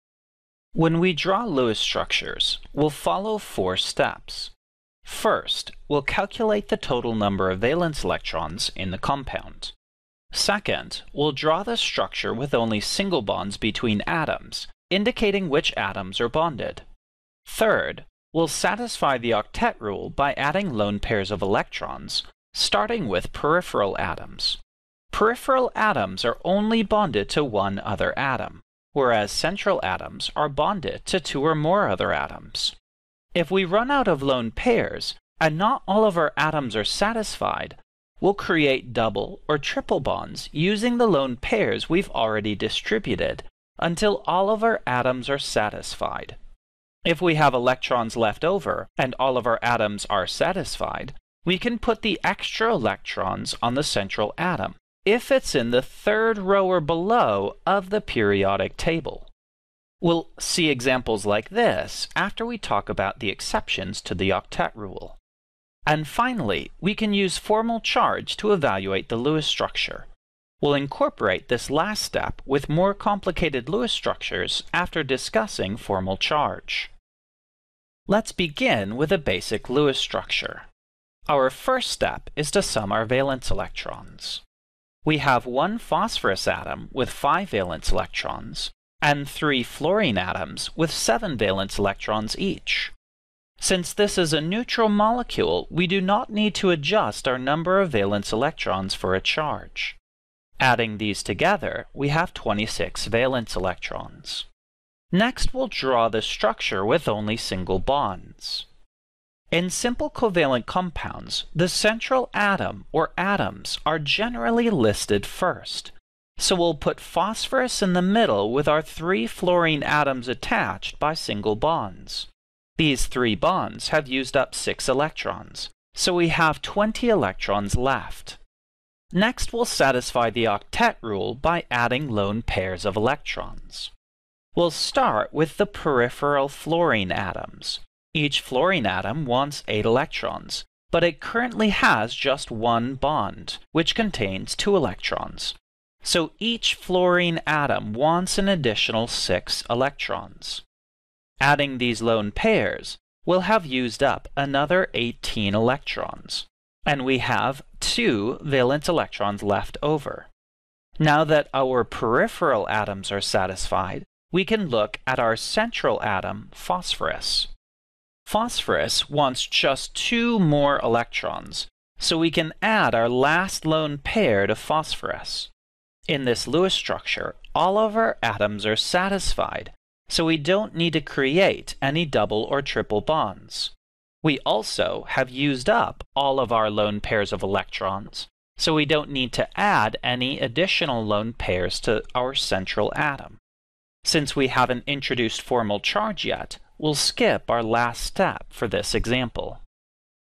CHEMISTRY 101 - Draw Lewis dot structures for covalent compounds
Learning Objective:  Draw Lewis dot structures for covalent compounds

Topics:   Lewis structures